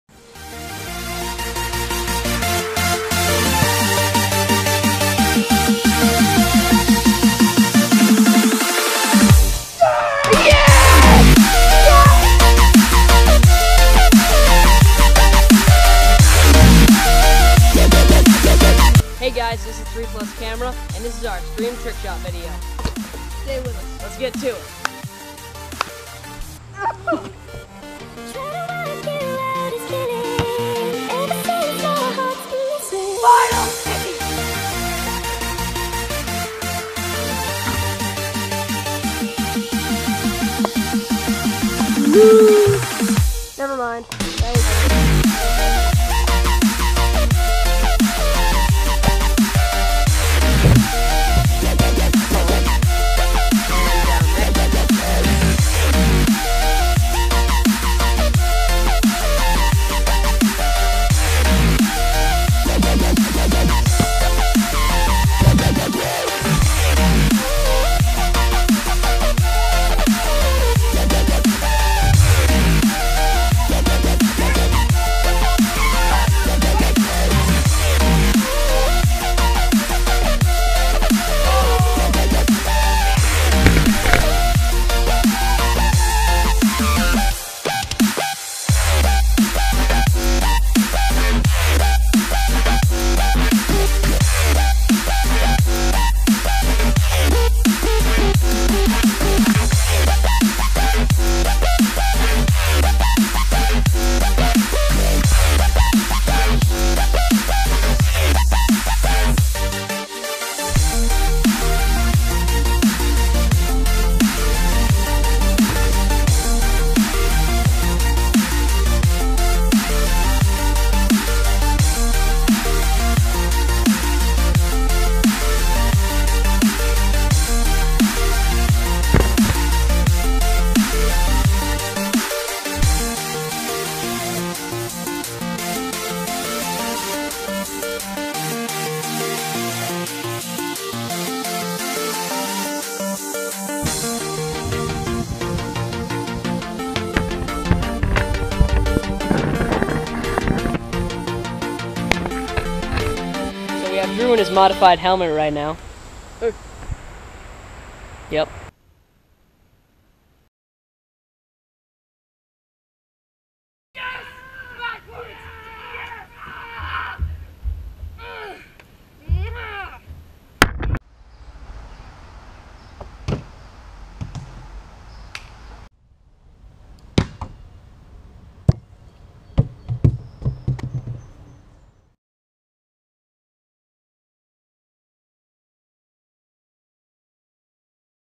3+ Camera's Extreme Trick Shots
Just some Summer Fun in the Sun (and the clouds). Some cool Basket Ball, Ping Pong, Bow and Arrow and Frisbee trick shots, with no actual hoop...